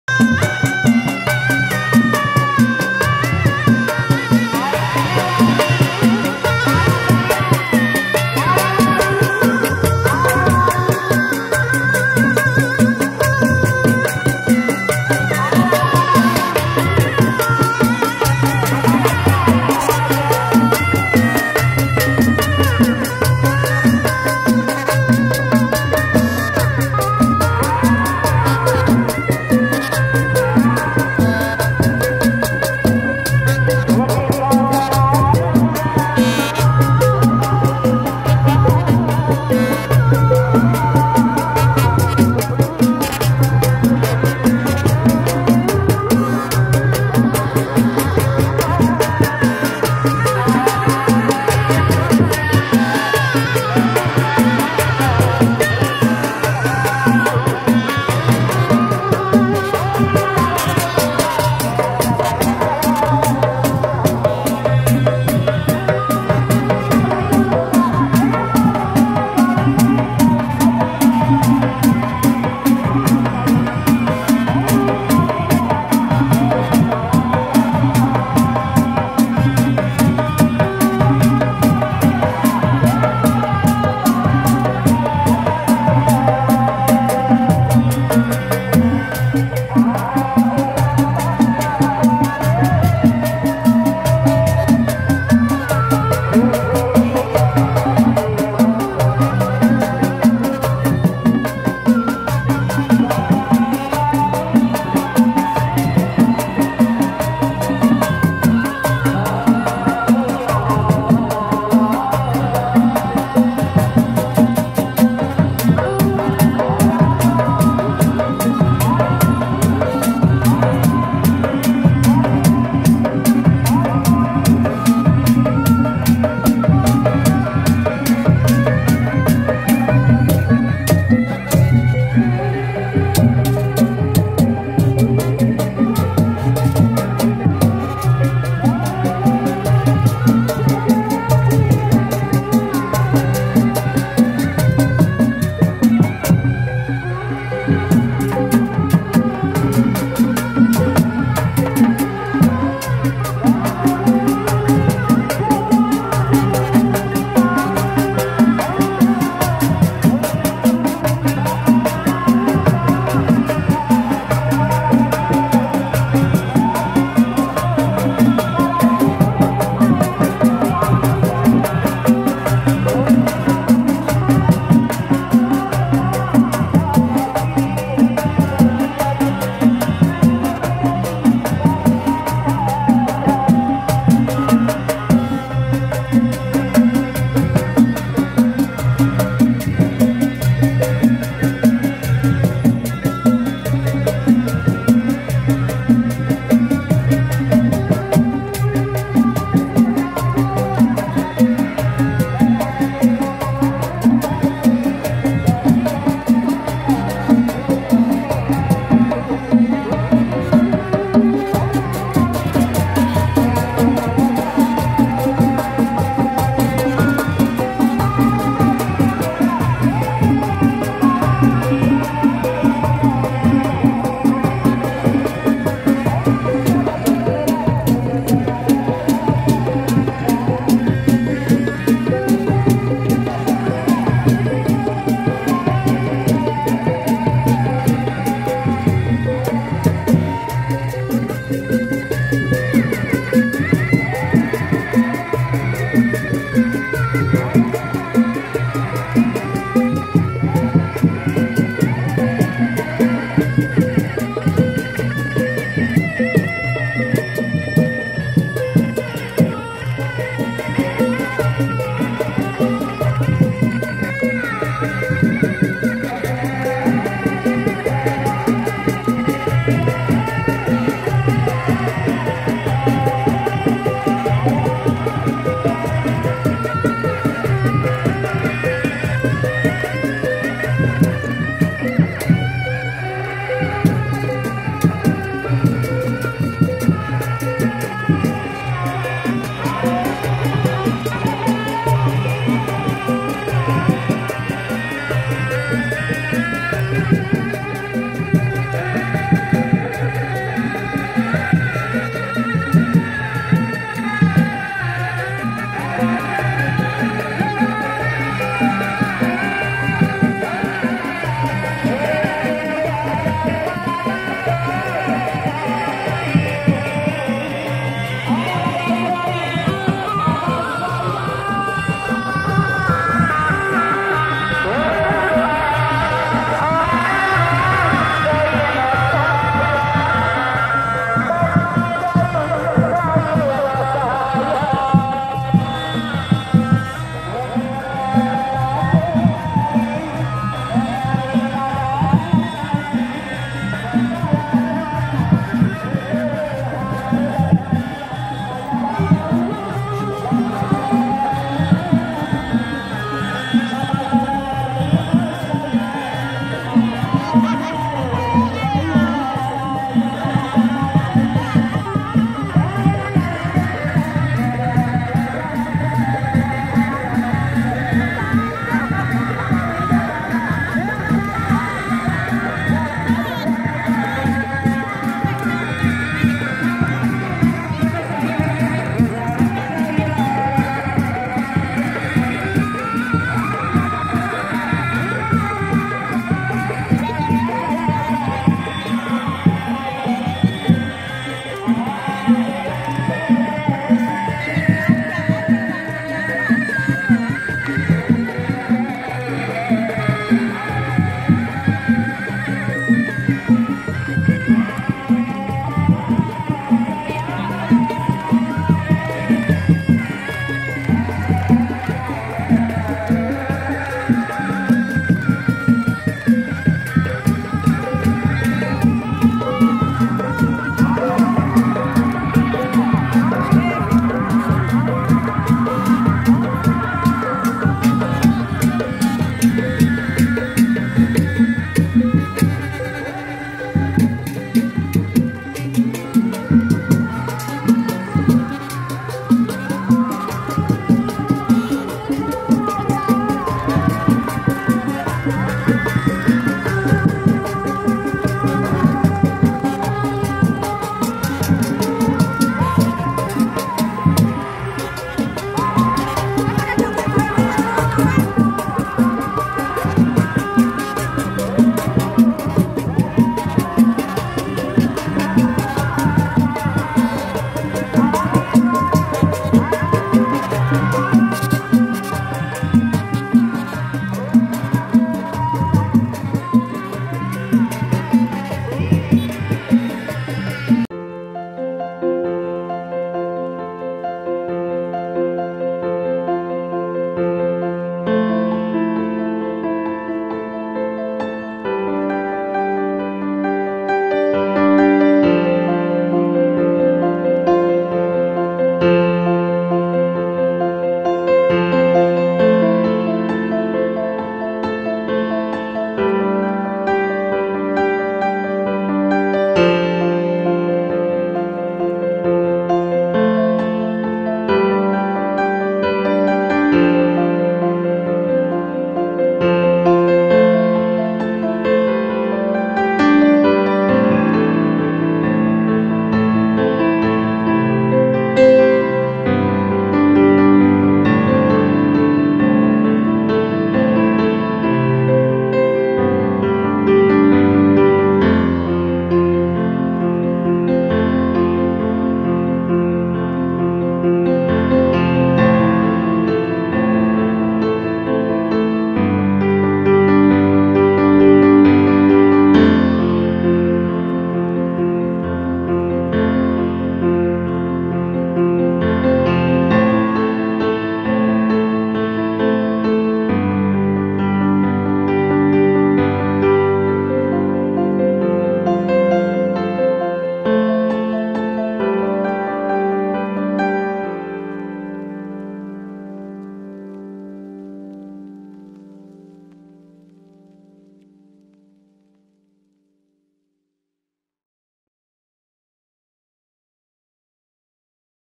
KUDARENGGONG BAYU & MEKAR RAHAYU GROUP BIKIN CERIA BOLANG 1
Assalamualaikum Wr Wb, Halo Semuanya, Salam Sehat Semoga Ada Dalam Lindungan Alloh  Swt,Tuhan Semesta Alam, Amiin, Saya berkesempatan  menyaksikan Kuda renggong Di Kp Bolang 1 TanjungsiangJuga Di Iringgi Ls Mekar rahayu Group Buah Dua Sumedang Yang Bikin Penontonya Bergoyang,Sangat Meriah Juga Sangat Terhibur,Yang Pastinya Seru, Tonton Terus  Videonya Jangan Lupa ,subscribe ,Like & Share Juga Coment  Trimakasih